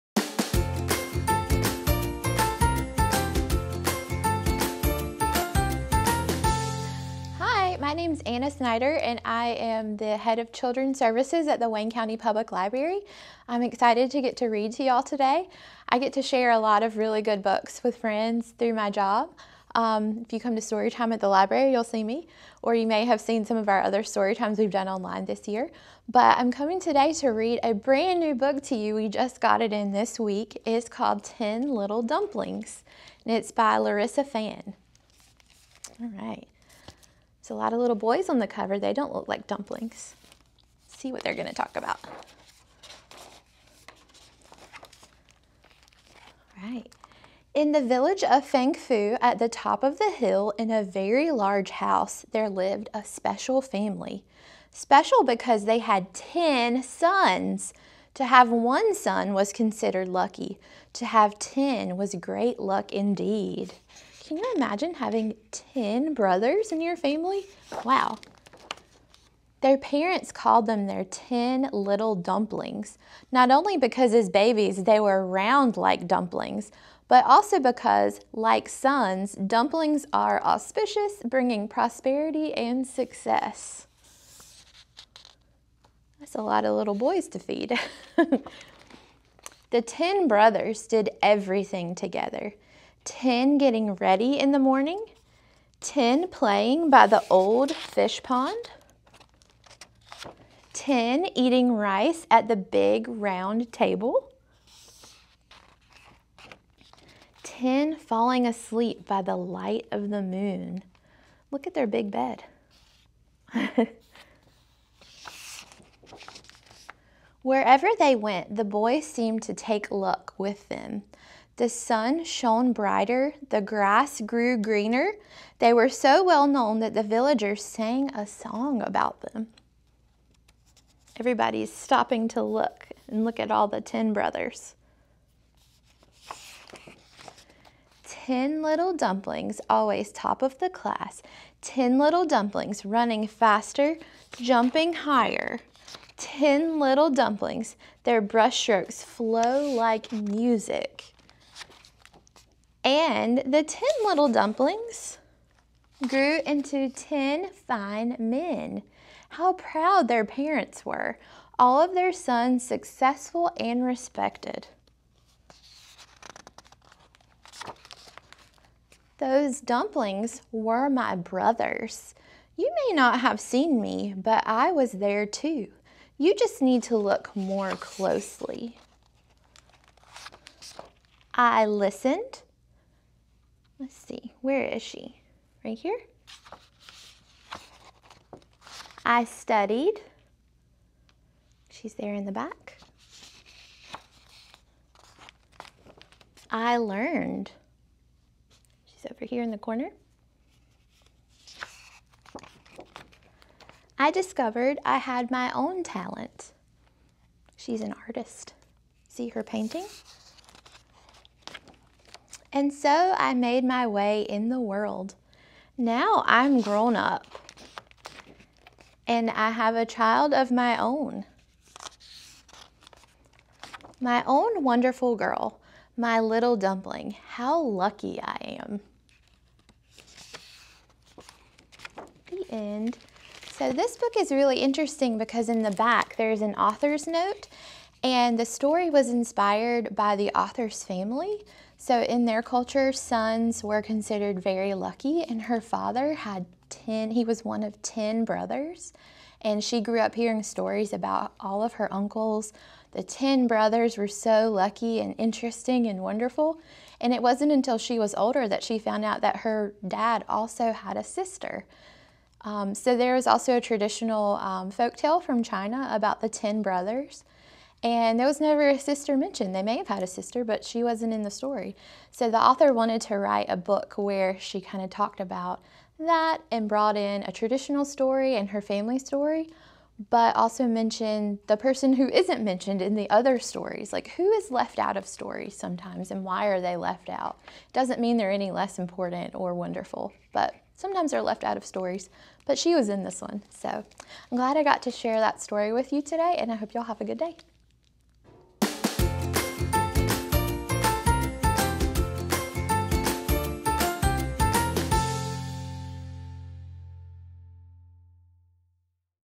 Story Time Online with Anna Snyder reading Ten Little Dumplings
Anna Snyder, the Head of Children’s Services at Wayne County Public Library is back with us again to read to us and share some of the super fun activities going on this summer at the library.  The summer reading program theme is Tails and Tales where your child will learn all about animals and where they live and the cool things they do.  Summer Reading runs from June 21st through the last week of July and includes some very special performances for children birth through 12.  To learn more about the program and to register your child, Anna explained that they have really fun activities planned four days each week by age.  They have an evening session and also virtual programs.  Come join their garden program and summer reading at the Cliffs of the Neuse.  There is something for every child.  Anna says lots of new books are arriving at the library all the time.  All you need is a library card, and that is easy to get.  Anna wanted us to remember that whatever we as adults enjoy doing, kids love to help us.  Take time this summer to slow down and let your little ones get involved with the things you do.  Anna’s daughter likes to help with the cooking, so they watched a video together about cracking eggs and making muffins, and now her daughter likes to be the one to crack the eggs when they cook together.  She also encourages us to play outside and work in the garden.  Hands-on learning is a great way to learn. Of course, bring home lots of books and work on any tip sheets your child’s teacher may have sent home.  Most of all enjoy your summer, and come visit Anna at the library!  Sweet Dreams!